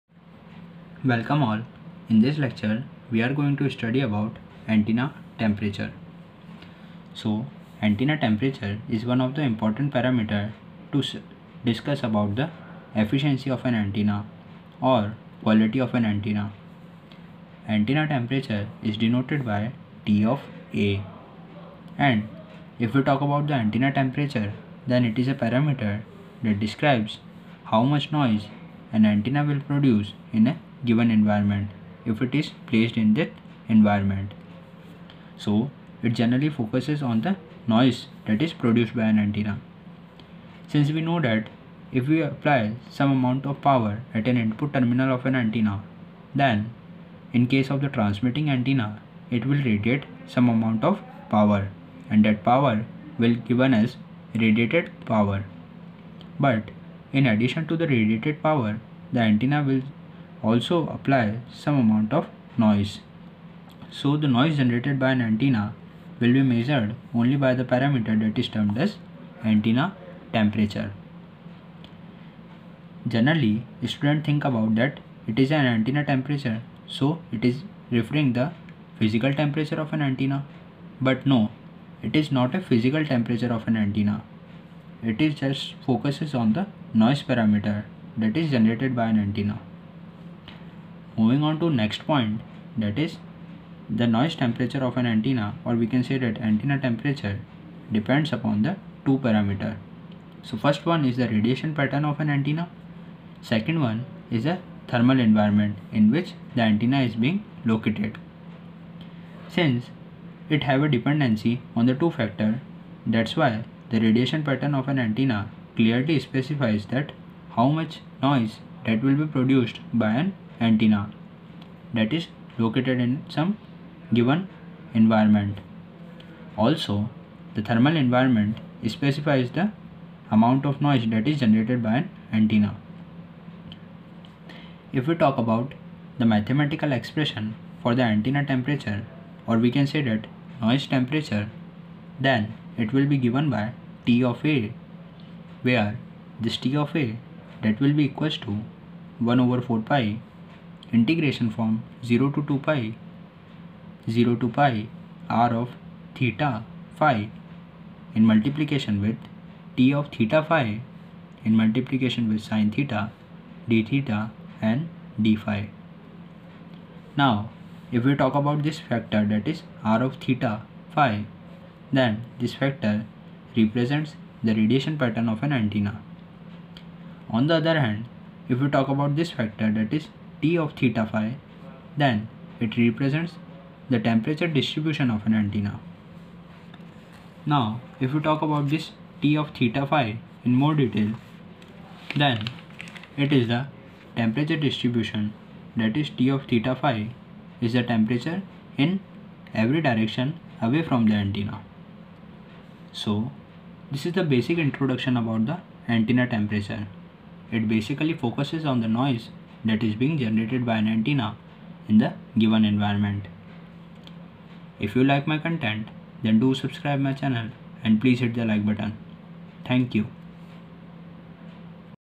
Antenna Temperature | Radiation Parameters of Antenna | Antenna and Wave Propagation
RTU Wallah channel covers subjects specifically designed for Rajasthan Technical University .
Here, this video is a part of Radiation in Electromagnetic Theory.